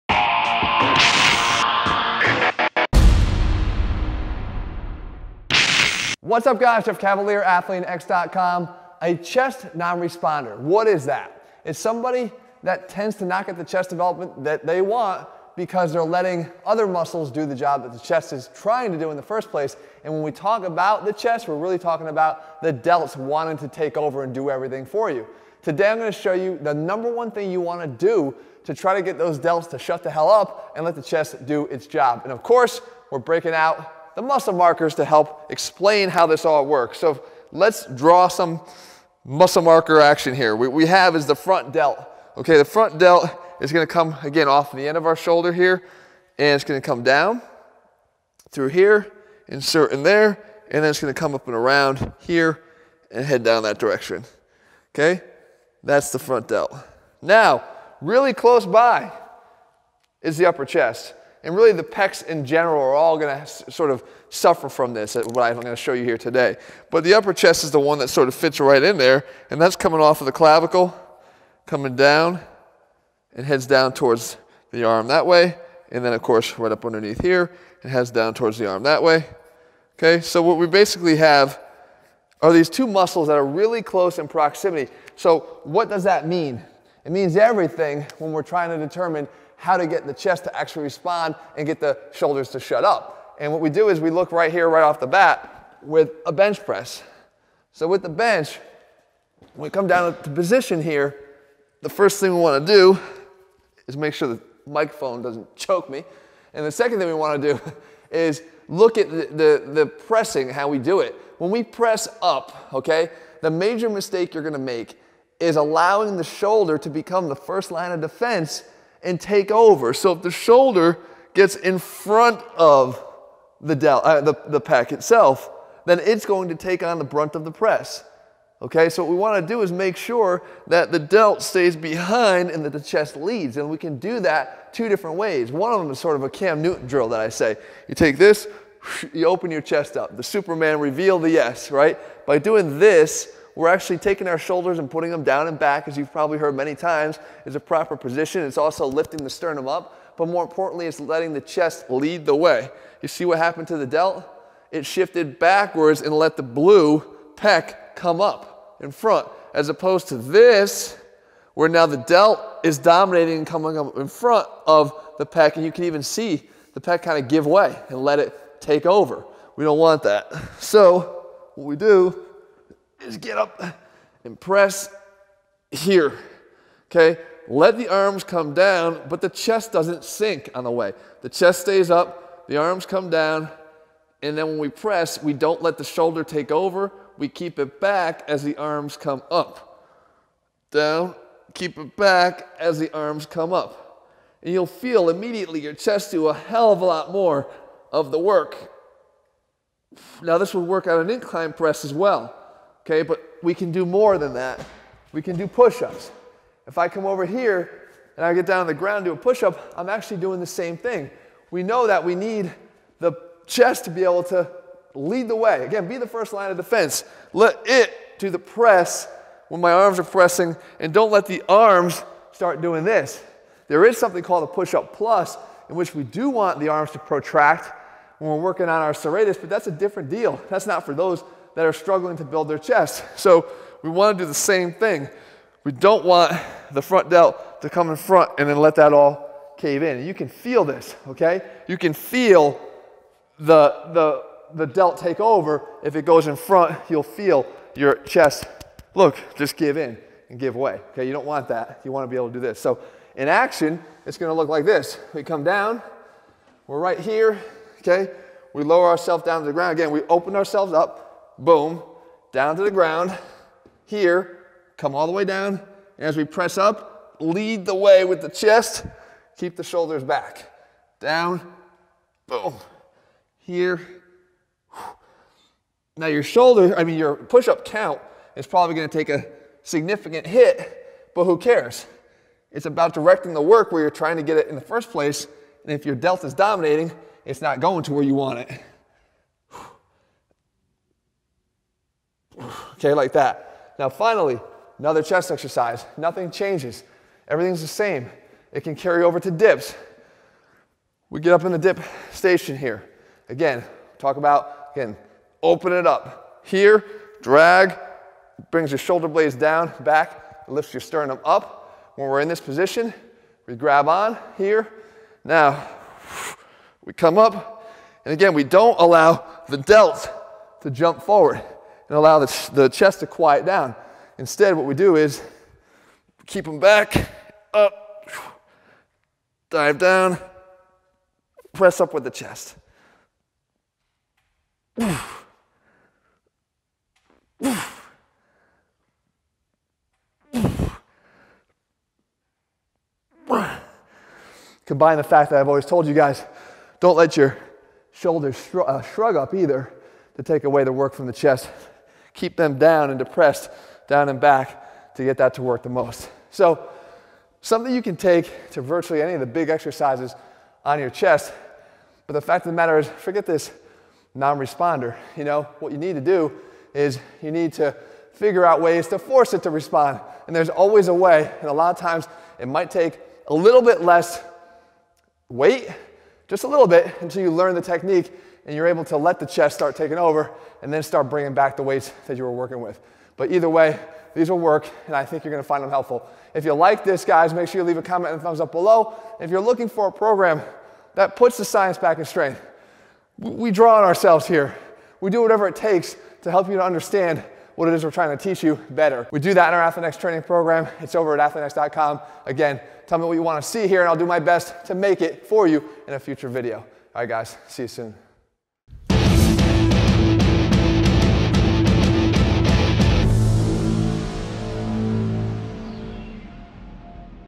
Chest “Non Responder” Solution (PECS WON’T GROW!)
Get a bigger chest with one program

Having chest muscles that don’t respond to normal training and resist growth is actually pretty common.  The reason for this however can be boiled down more to the way you perform your chest exercises in your chest workouts than it can on your genetics, and that’s a good thing.  In this video, I’m going to show you one of the most common mistakes made in chest workouts that is limiting the response of your pecs to the exercises you are doing for them.

To illustrate this, literally, I am breaking out the muscle markers to show you why this occurs.  When you look at the anatomy of the chest you quickly realize just how close in proximity and function the front deltoid is to that of the pectoralis muscle.  Because of this close relationship, there is a tendency of the deltoids to want to dominate the movements that are supposed to be performed primarily by the chest.

Look no further than the classic bench press exercise.  When you lie either in the flat bench or incline bench position, the setup of your shoulders in relation to the chest is key.  You have likely heard that you need to keep your shoulder blades pinched together and your shoulders down and back.  This doesn’t just apply to the beginning of the movement but throughout the movement.  This is critical because doing this provides not just stability to the shoulder joint but it allows the pecs to be the main drivers of the exercise.

I demonstrate this with the use of the markers and by drawing out both the front deltoid muscle and the upper pec fibers.  When you press up, if you allow your shoulders to protract forward during the press you will see that the deltoid moves into a more prominent or forward position in relation to the pecs.  This is a mistake.  When placed in this up front position, they are more equipped to take on the load of the chest exerise that you are performing and the delts will quickly dominate  the movement.  While this is good for the hypertrophy of the shoulders it is taking away from the development you are seeking in your chest.

Instead, you must make sure that the first line of defense in any movement is the chest.  You can assure you are doing this by keeping those shoulders down and back, but more importantly, lifting your sternum up and sticking your chest out at the same time.  If you can always keep your chest more proud of your shoulders, then you will be assuring yourself that the chest is taking on the brunt of the chest exercise while the shoulders are relegated to the more supporting role they should have.

I show you how this applies to not just the bench press and incline bench press exercises but virtually all of the major chest exercises.  In the bodyweight equivalent, the pushup, the same thing applies.  Most of us tend to push up and allow our chests to sink back as the arms press away from our body and the shoulders drift in front of the chest.  Do it instead the way I’m showing you here and while you may find that you can do fewer pushups, you will be getting much more out of them in the process.

Same thing goes for dips as well.  On this exercise however, not only are you going to get more activation of the non-responder chest muscles but you are going to be putting your shoulder in a much safer position.  Doing dips while allowing your shoulders to round forward is a recipe for shoulder disaster that you want to avoid at all costs.

So you can see, before you start blaming your genetics for why you can’t build a bigger chest first look to your form and see if you aren’t doing what I am showing you here.  Fix it quickly and then look to take your results to the next level by applying the same critical evaluation of everything you are doing in the gym.  is designed to help you to get the most out of your training by getting you working out like a pro athlete.